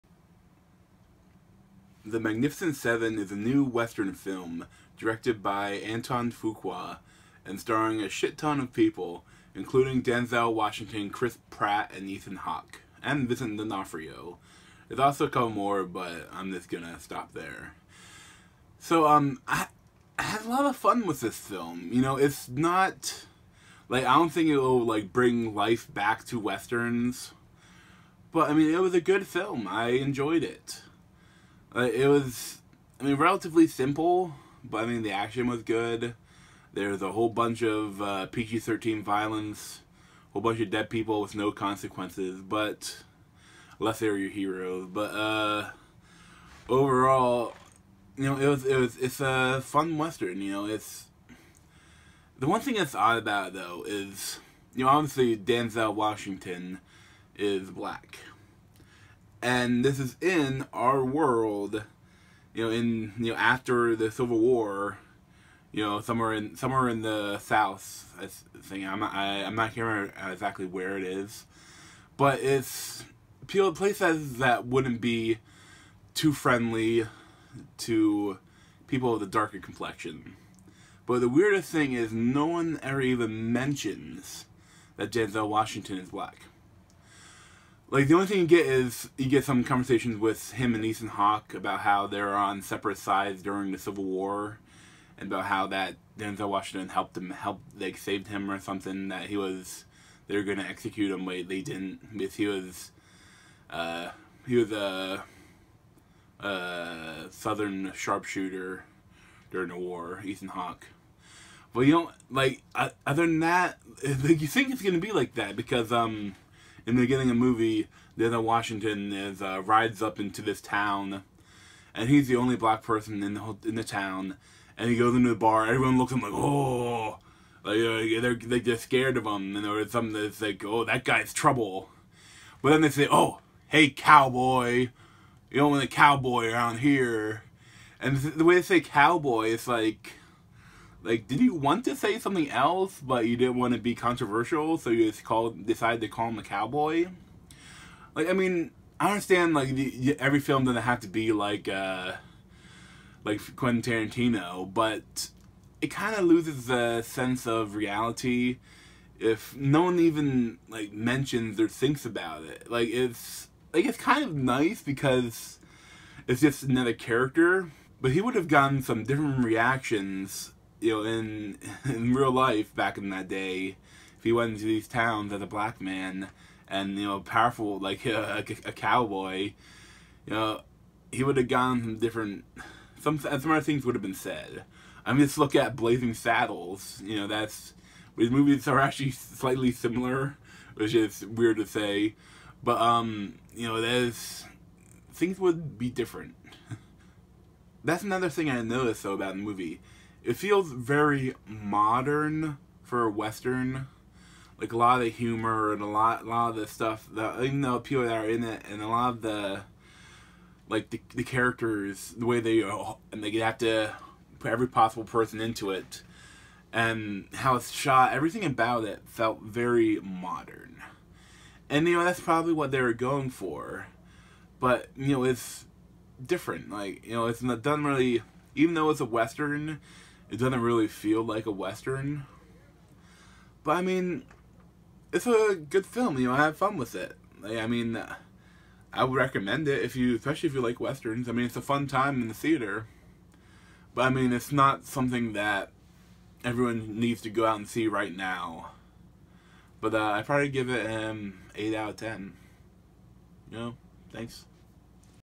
The Magnificent Seven Movie Review
This is the fifth in a series of relatively quick movie reviews, and I'm now caught up with watching and reviewing all of the movies I wanted to see that are currently out.